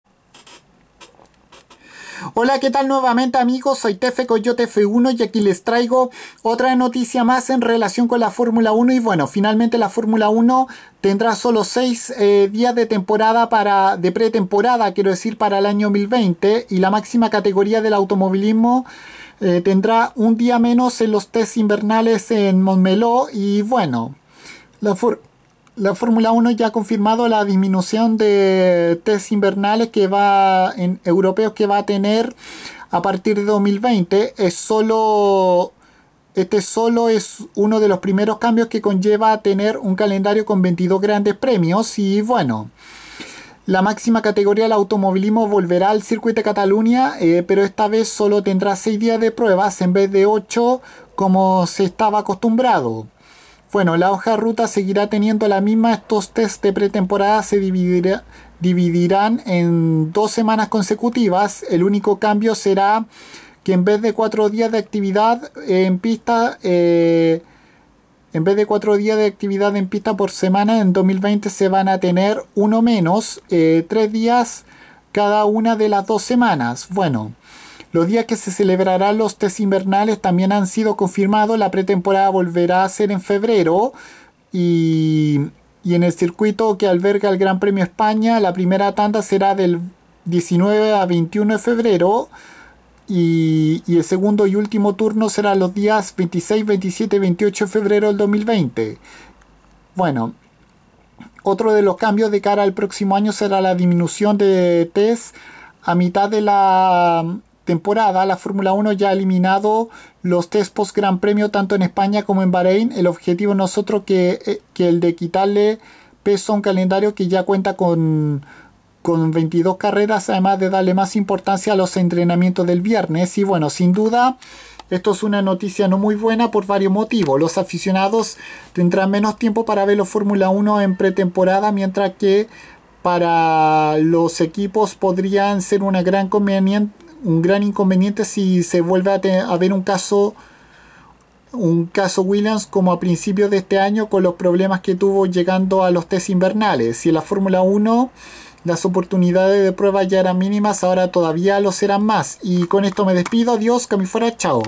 La pretemporada 2020 de F1 tendrá un día menos
Se volverán a hacer en Febrero próximo y ademas solo tendrá 2 semanas diferentes también se confirma que se volverá a hacer en el Circuit de Catalunya Barcelona(misma pista que actualmente es la sede del GP de España)